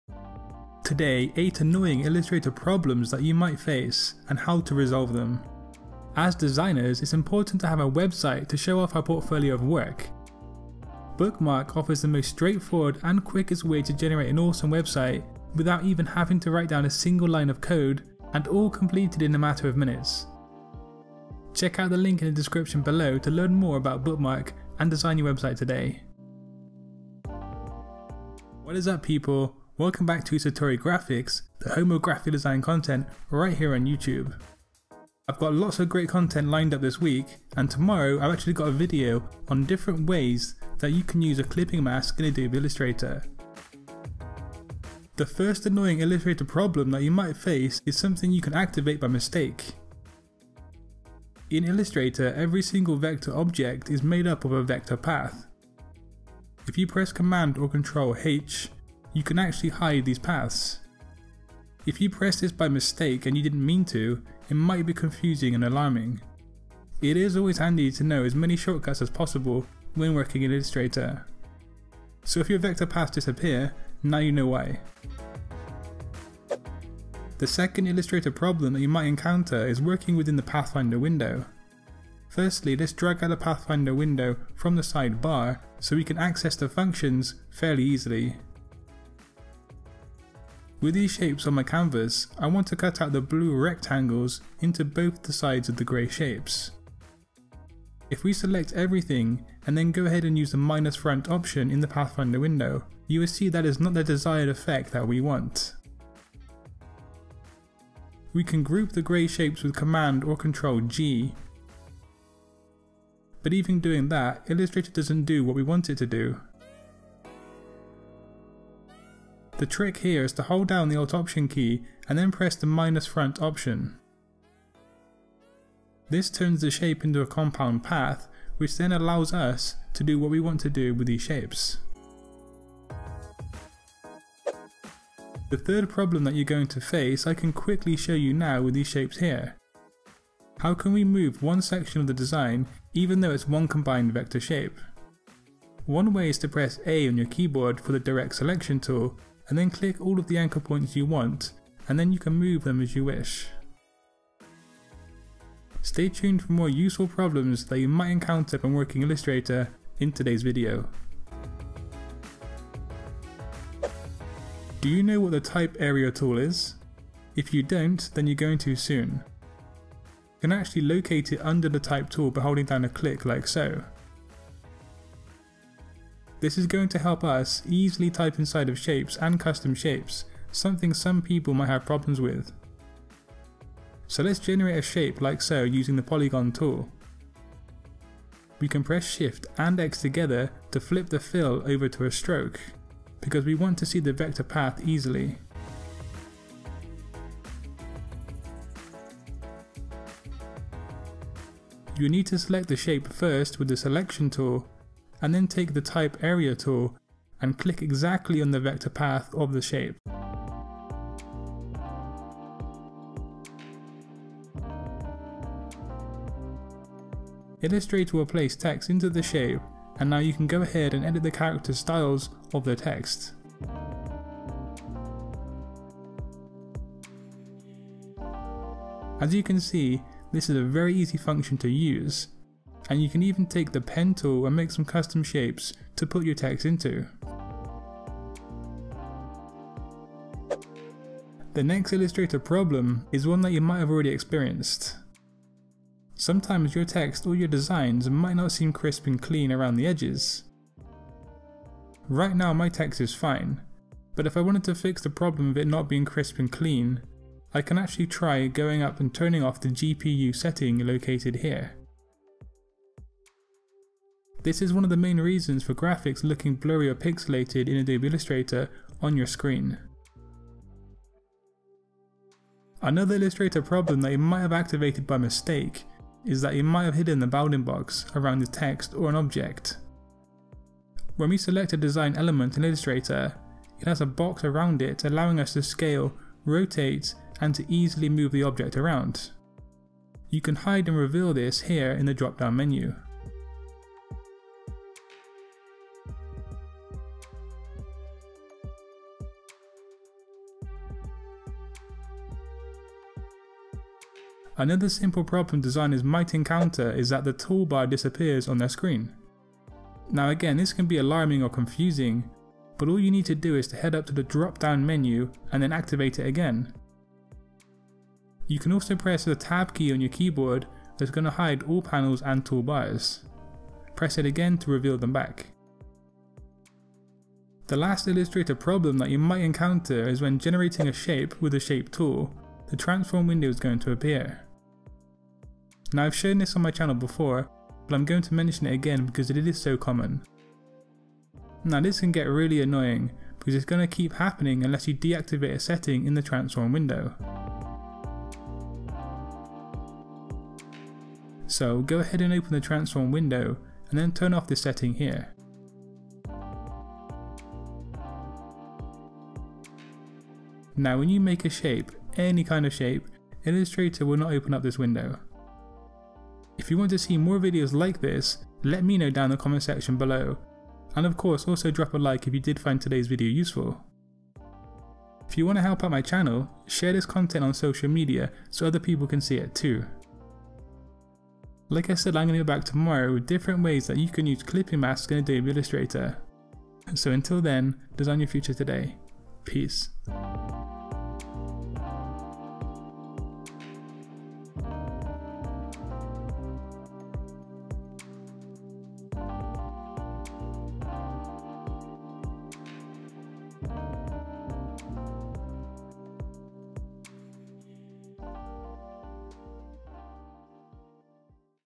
ANNOYING Illustrator Problems ((SOLVED))
Do you experience these 8 Illustrator problems, or maybe you have done in the past? Well today I have the solutions to all of the 8 potential problems yo may face working in Adobe Illustrator.

There are many weird problems that designers can face in Illustrator, and often there is a simple and quick solution to that problem, quite a lot of the tie a change in setting choice. These 8 problems are not bugs of Adobe Illustrator, but more issues where there is a lack of knowledge about the functions, tools and settings of Illustrator.

If you found this graphic design upload on 8 annoying problems in Illustrator enjoyable or useful, let me know in the comments section and drop a like on your way out.

0:29 to skip the intro

MUSIC